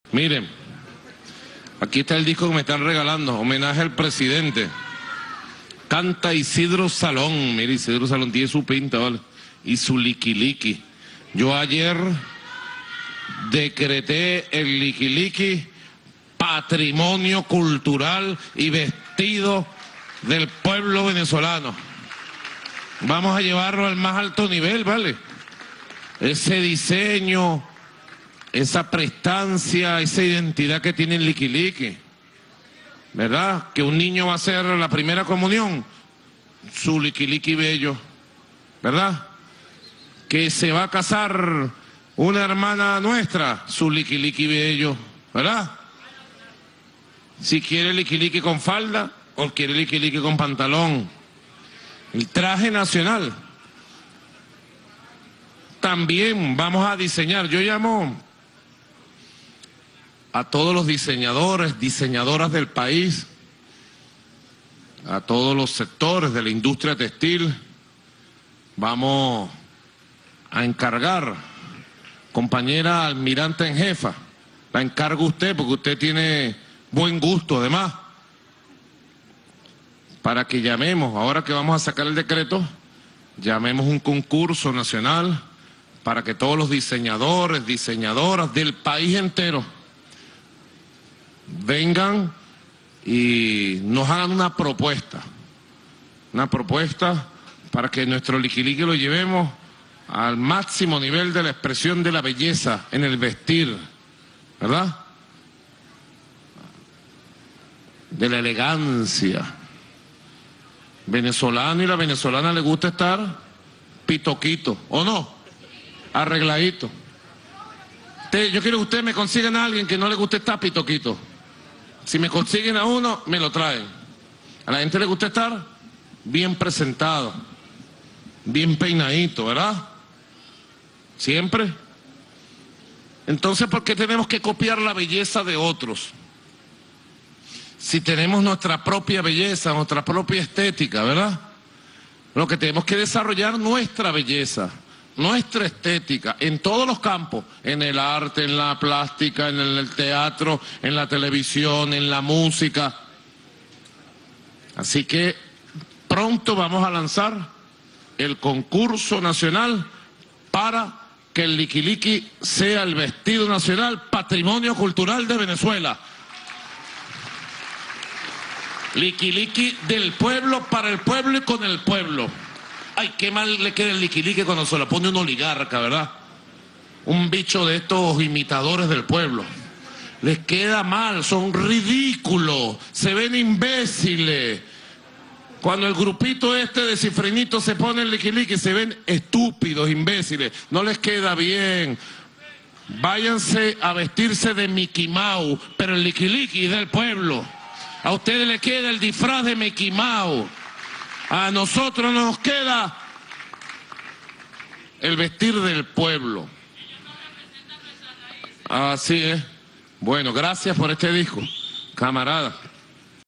Presidente Maduro convocará a diseñadores para concurso nacional de liquiliquis
El Jefe de Estado impulsará el uso del liquiliqui (traje llanero nacional) en todos los ámbitos, y lo convertirá en patrimonio cultural de Venezuela. Acto por los 18 años de la instalación del gobierno del Presidente Hugo Chávez, 2 de febrero de 2017, Palacio de Miraflores.